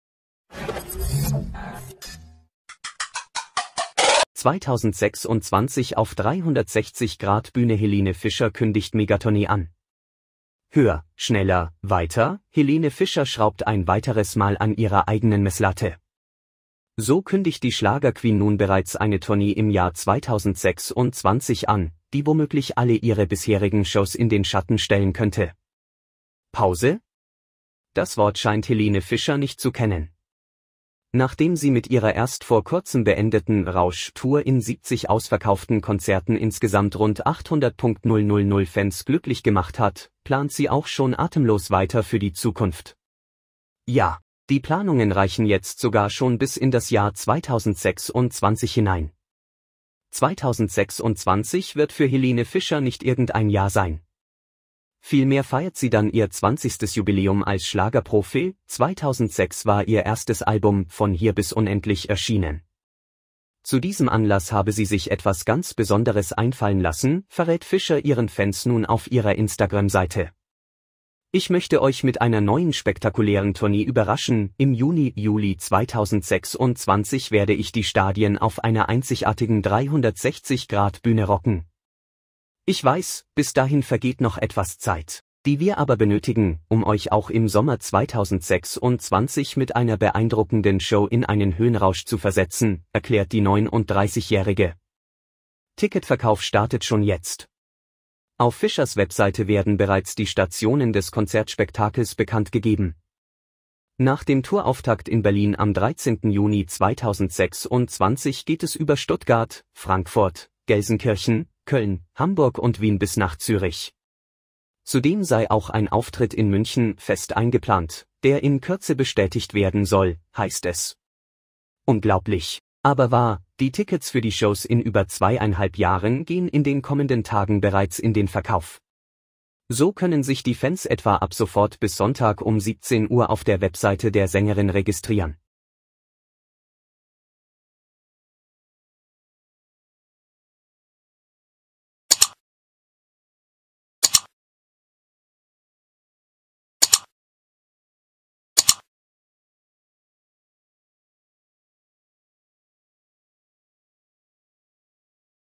2026 auf 360-Grad-Bühne Helene Fischer kündigt Mega-Tournee an germany | SH News German
2026 auf 360-Grad-Bühne Helene Fischer kündigt Mega-Tournee an germany | SH News German